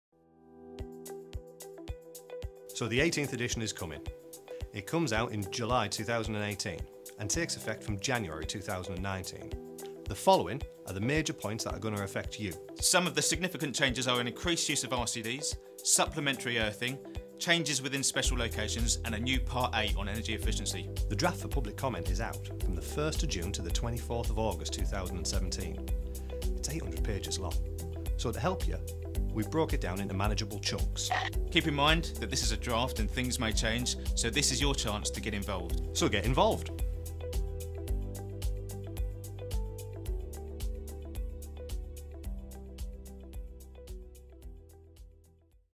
BS 7671 - Draft 18th Edition of the Wiring Regulations is Coming
ECA Technical team is introducing some of the major changes proposed to the Wiring Regulations and how these may affect our industry.
Project18 is an exciting new campaign from the ECA which aims to raise awareness of the 18th edition of the Wiring Regulations.